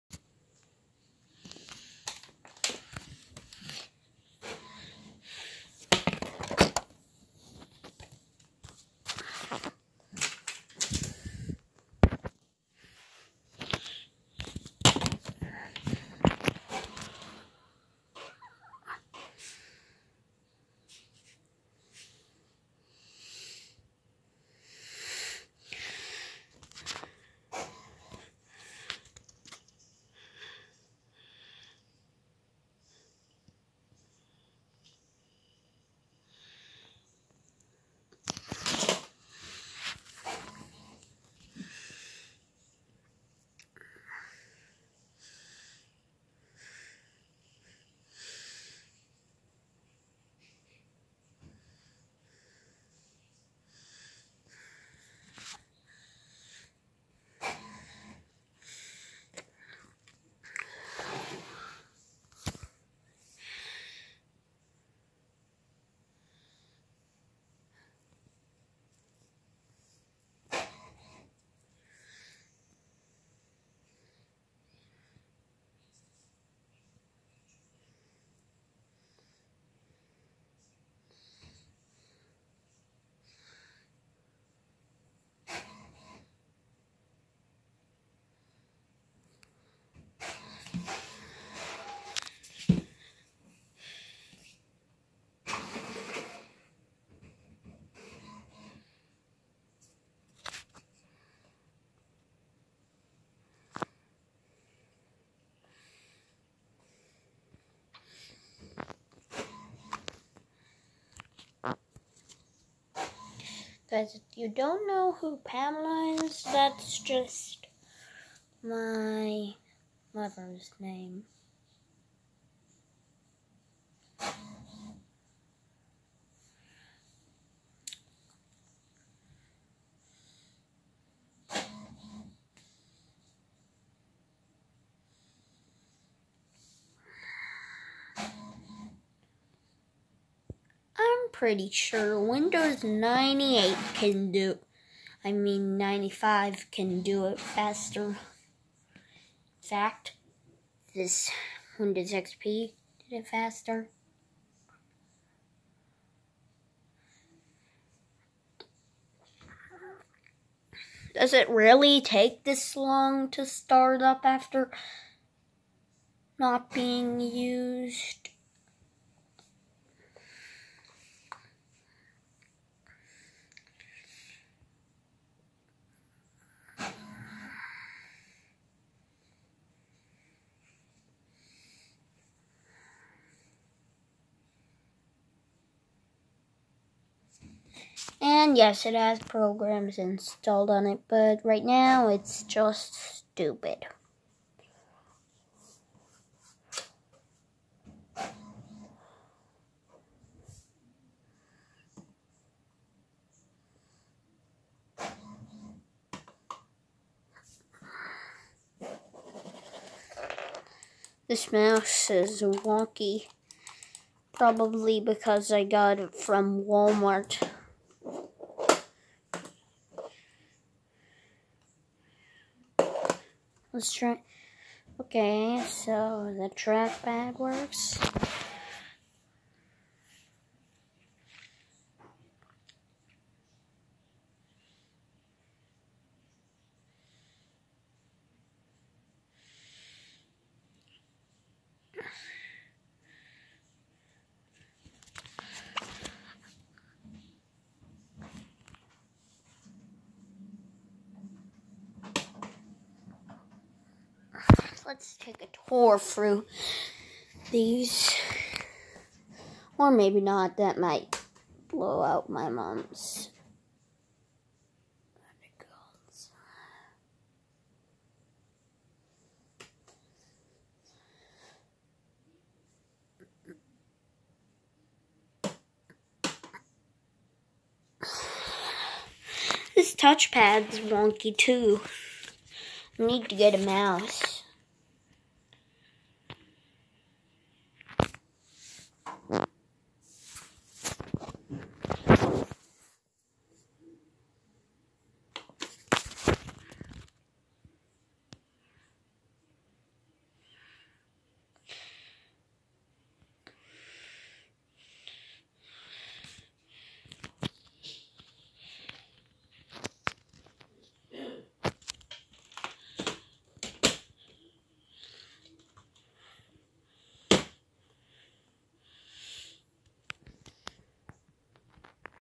2005 Dell Vostro 1000 (Windows XP Pro SP2) vs 2016 HP 14 (Windows 10) Speed Test
Specs:
Dell Vostro 1000
Windows XP Professional SP2
AMD Athlon 64 X2
Office XP
Windows Media Player 8
Internet Explorer 6

HP 14
Windows 10
Intel Pentium
Office 2012
Microsoft Edge

Any comments saying "This is not a fair test." will be removed.